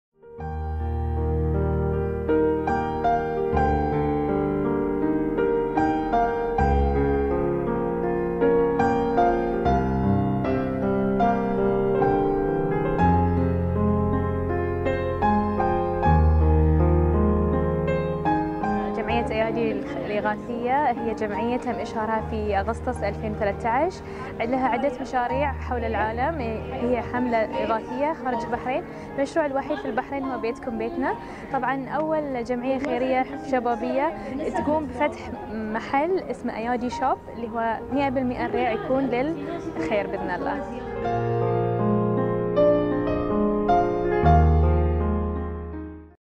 Made in Bahrain - صنع في البحرين (Ayadi Shop)
Today, we present to you one of the talents we met and want to shed light on: Ayadi Shop(@AyadiShop)

اليوم نقدم لكم إحدى المواهب البحرينية التي قابلناها مؤخراً
instagram (@AyadiShop)